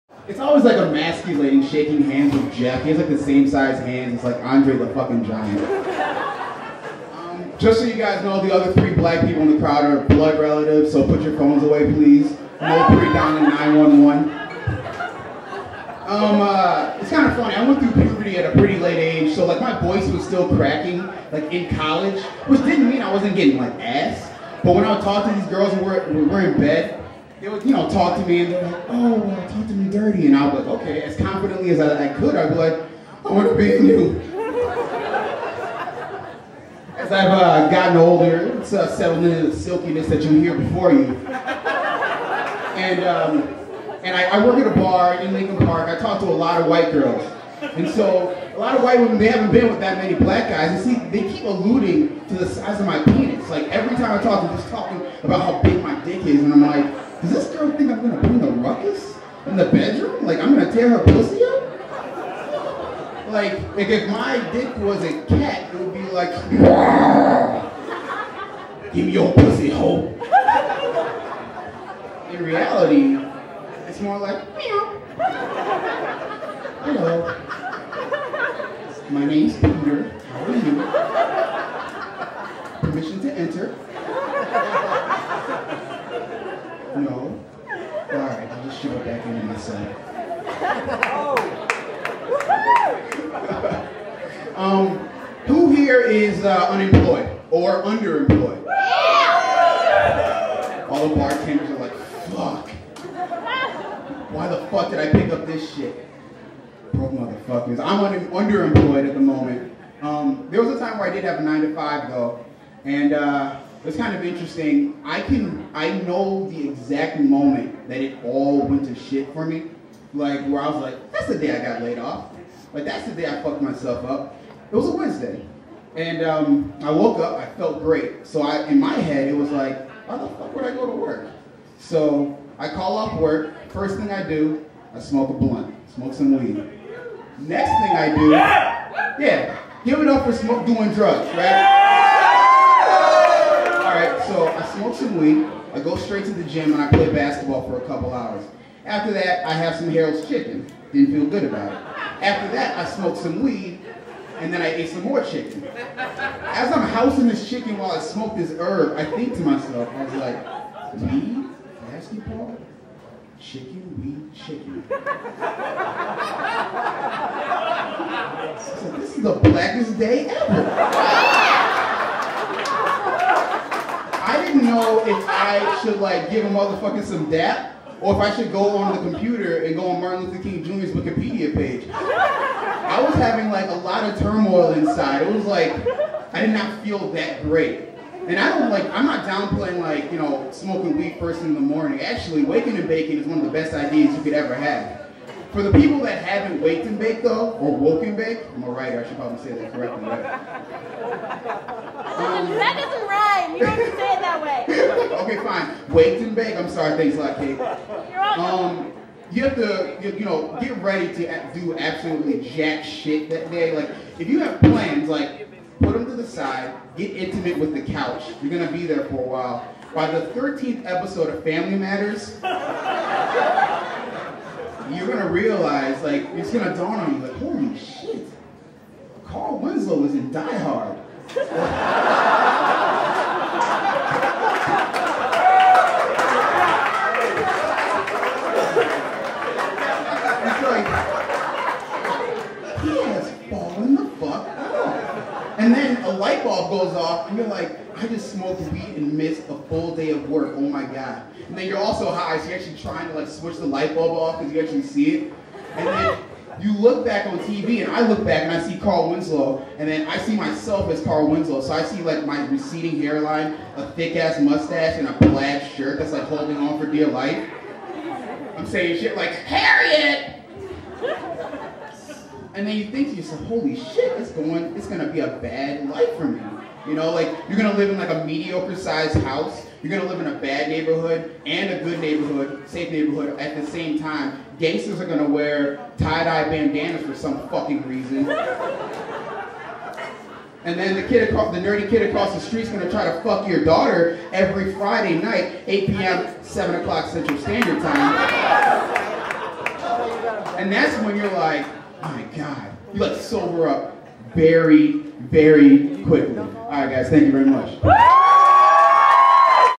Comedy Night - Summer 2010 - Rony
At Chicago Portfolio School, students give their final exams standing up.